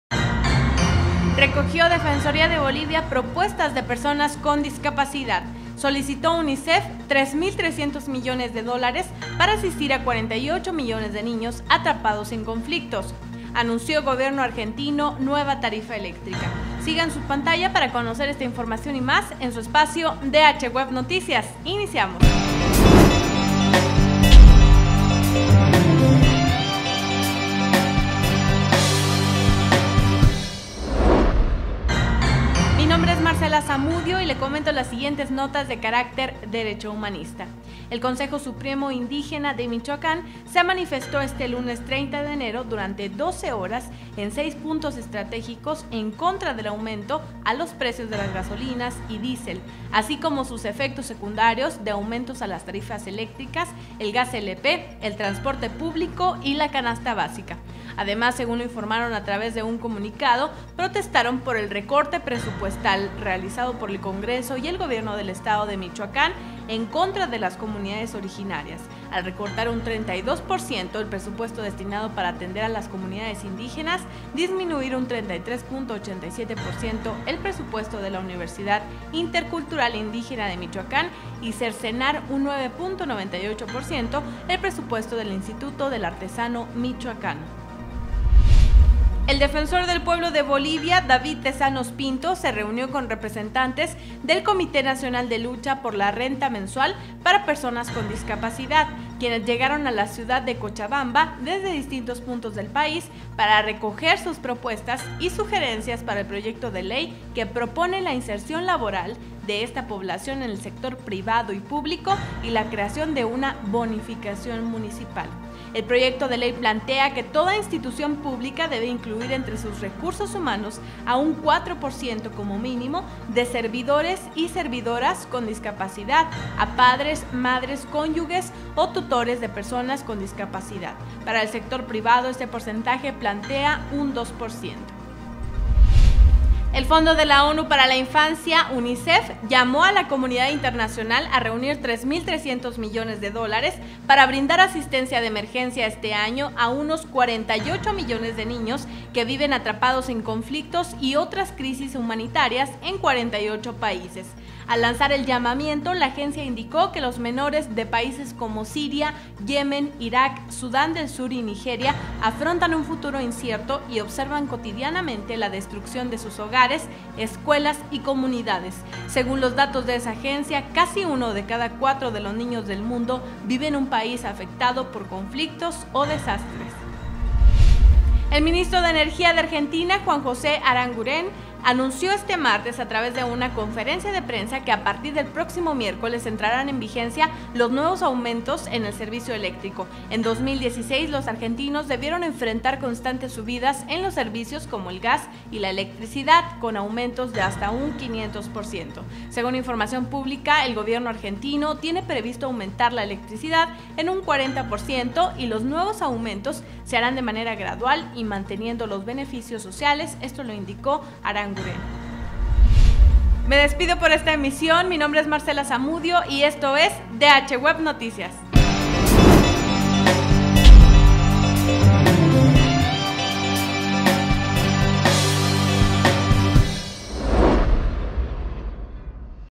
Se manifiesta Consejo Supremo Indígena de Michoacán contra Gasolinazo
- Recoge Defensoría de Bolivia propuestas de personas con discapacidad

- Solicitó UNICEF 3.300 mdd para asistir a 48 millones de niños atrapados en conflictos

- Anunció Gobierno argentino nueva tarifa eléctrica